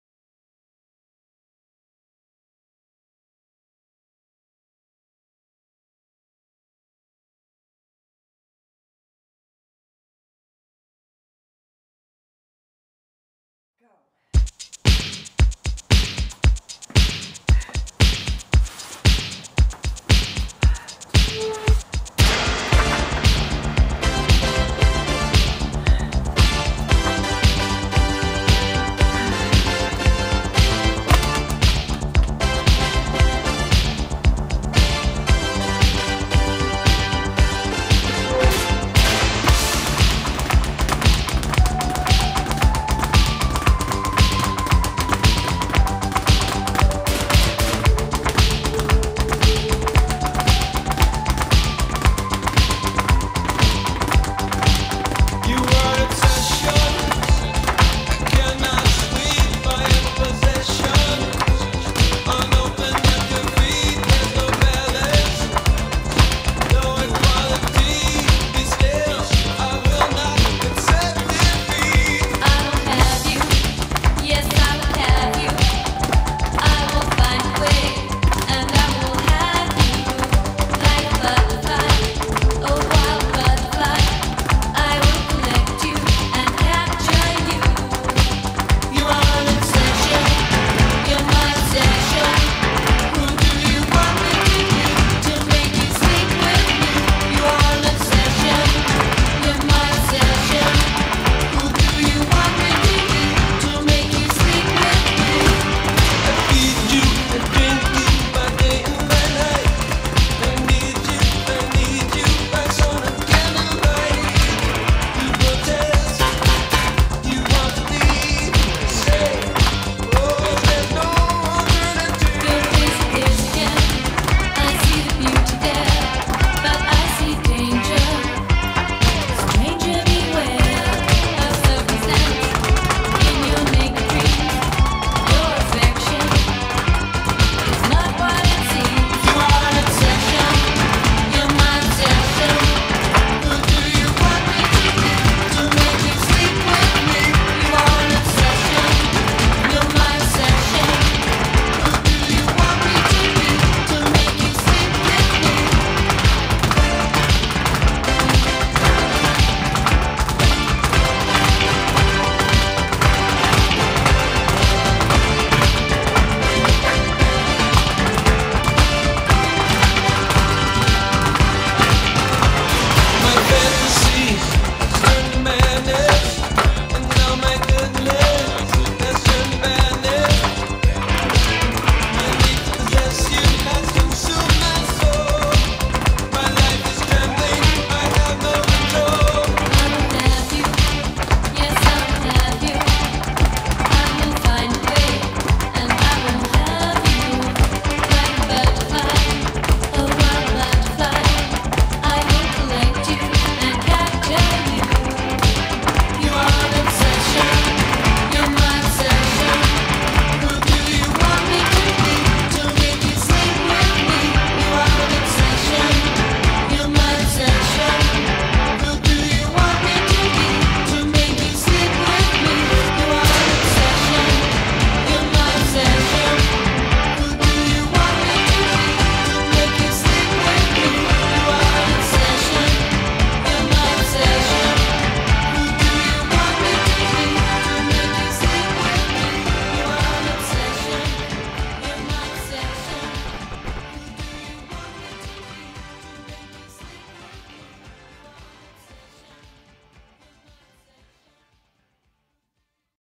Speed Bagging on Vertical & Horizontal | MsDeville's Speed Bag Obsession
Working on elbow combinations on both the horizontal speed bag drum as well as the vertical drum. Using a BFF Australian speed bag on the horizontal drum, and a Title 5x8 bag on the vertical drum.